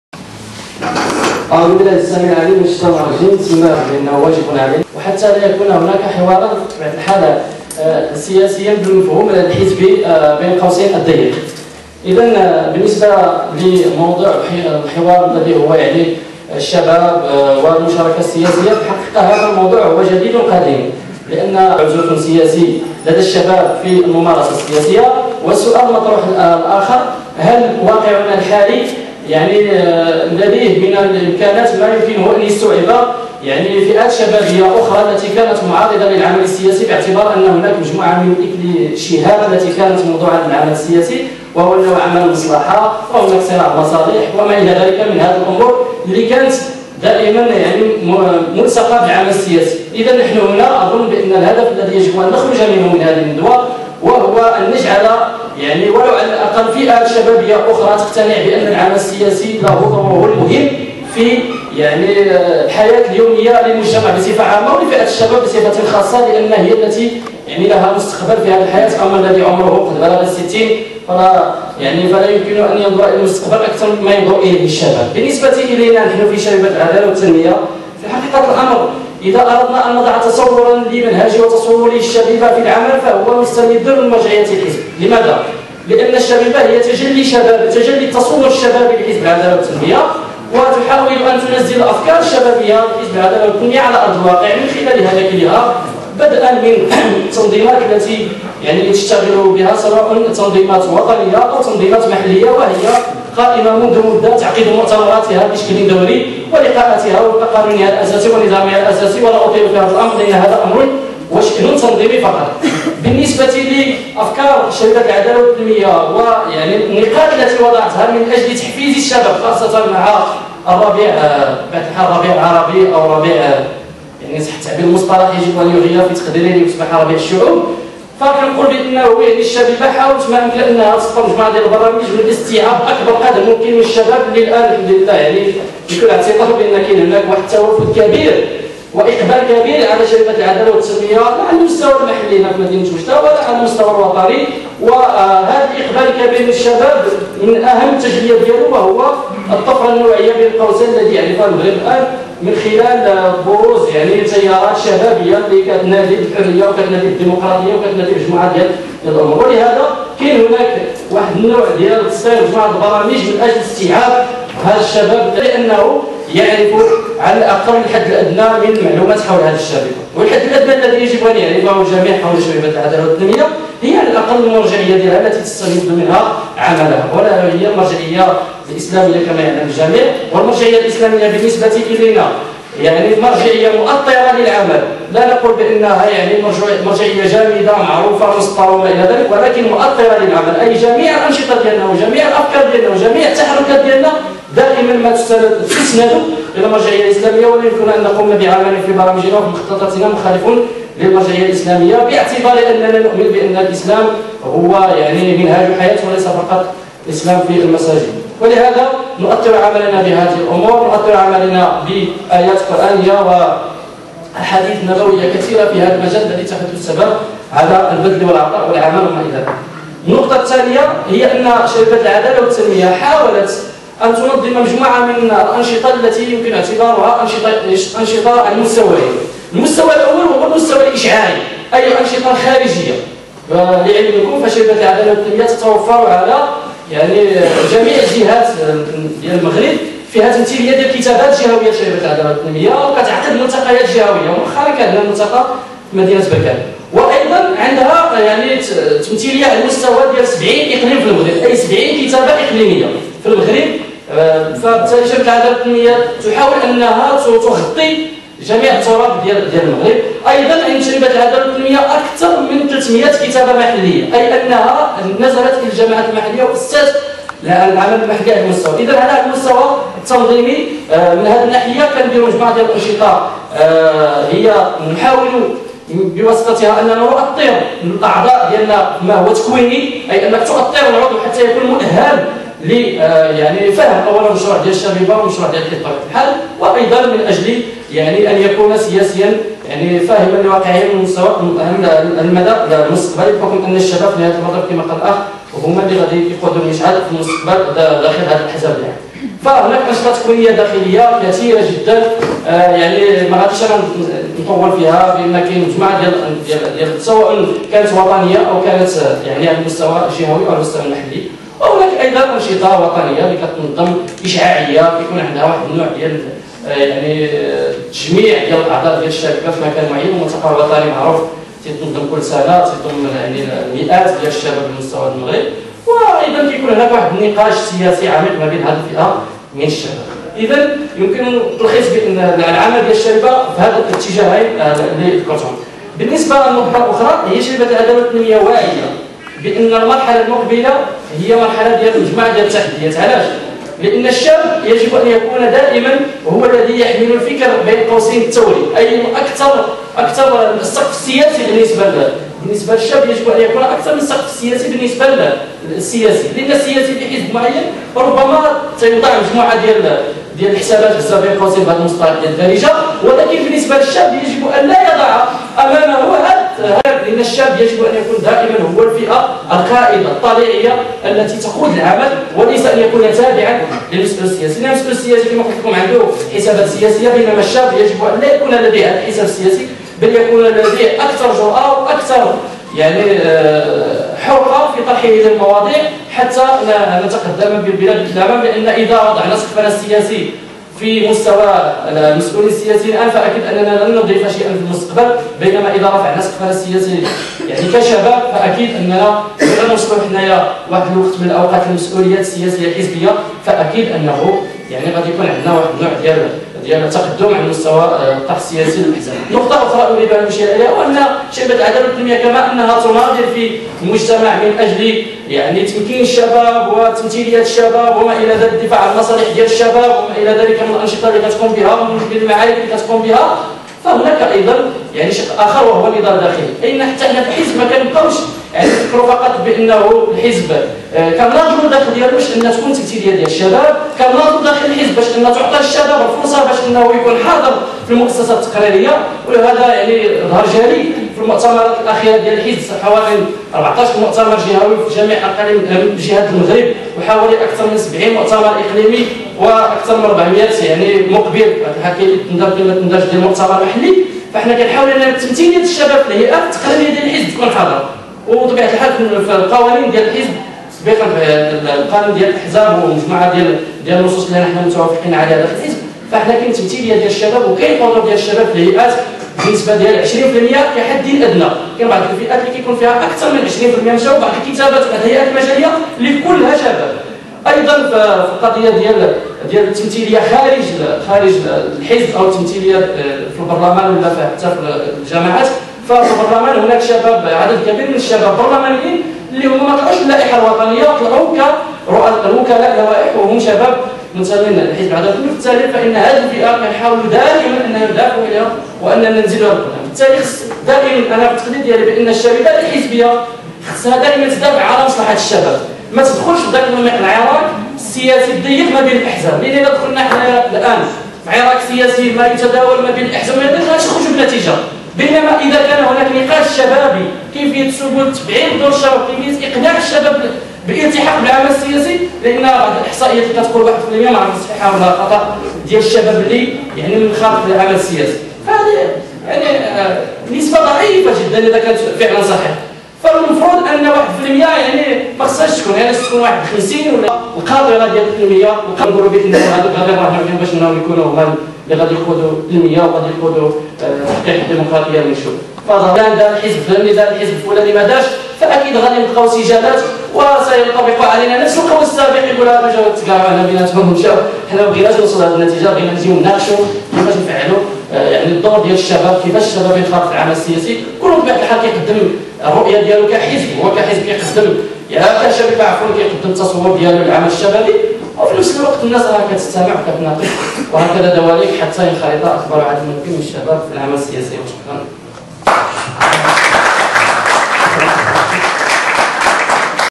رشيد الهلالي في الشباب والسياسة
تدخل رشيد الهلالي باسم شبيبة العدالة والتنمية في ندوة ازمنة سياسية حول الشباب والسياسة الواقع والافاق